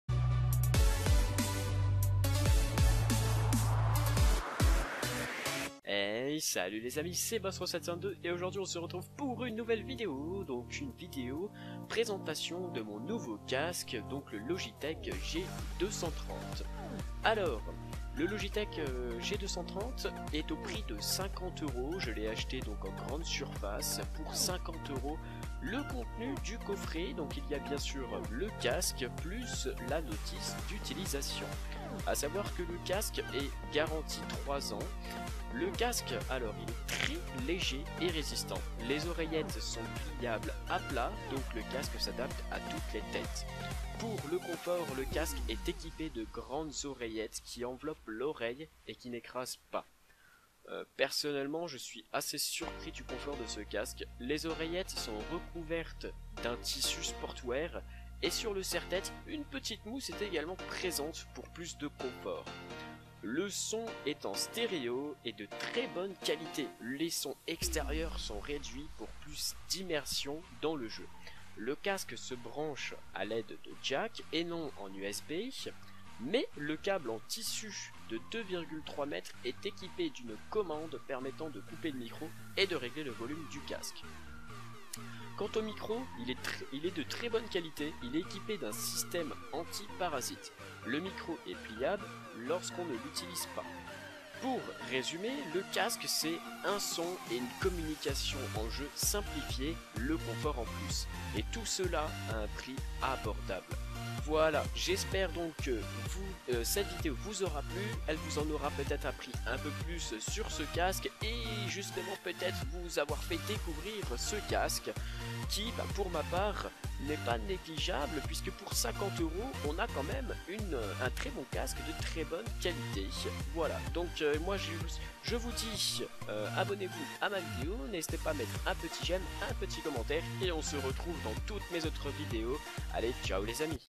CASQUE GAMING G230 PRESENTATION + TEST
Présentation d'un casque gaming de très bonne qualité le Logitech G230 pour 50€ !
♣ COMMENTE